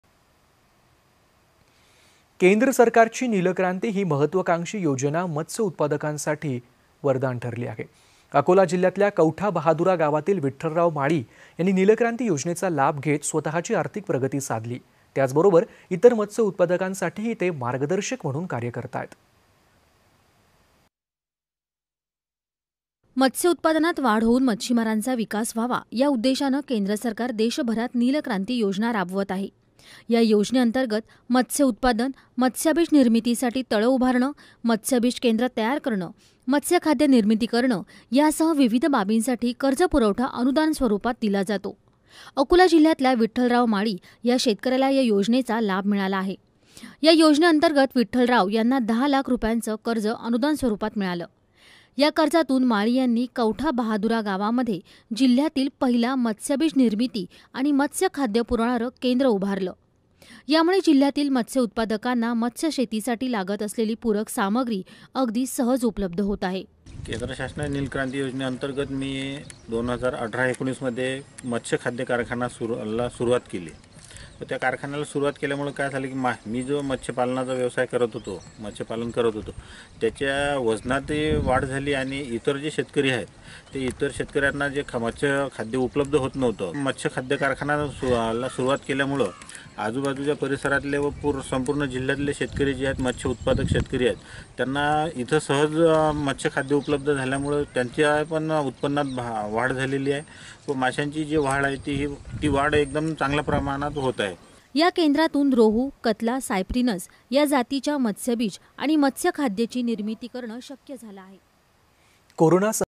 Vishal Bore DD News.शेतीला पूरक मत्स्यव्यवसायाची जोड,कवठा बहादुरा येथील विठ्ठलराव माळी यांची यशोगाथा
शेतीला पूरक मत्स्यव्यवसायाची जोड,कवठा बहादुरा येथील विठ्ठलराव माळी यांची यशोगाथा,अनेकांना प्रेरणा देणारी

@निलक्रांती योजना